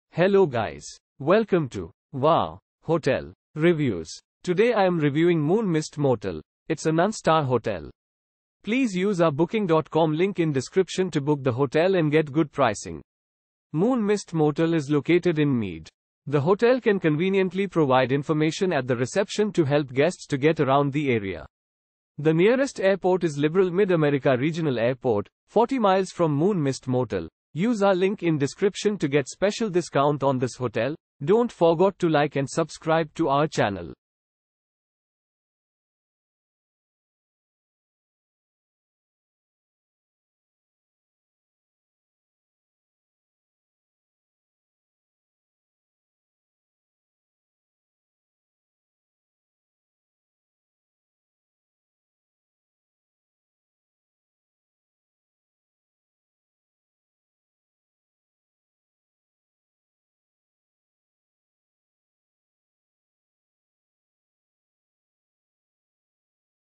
Moon Mist Motel Review - Meade , United States of America
Moon Mist Motel

Moon Mist Motel is located in Meade.
The hotel can conveniently provide information at the reception to help guests to get around the area.
The nearest airport is Liberal Mid-America Regional Airport, 40 miles from Moon Mist Motel.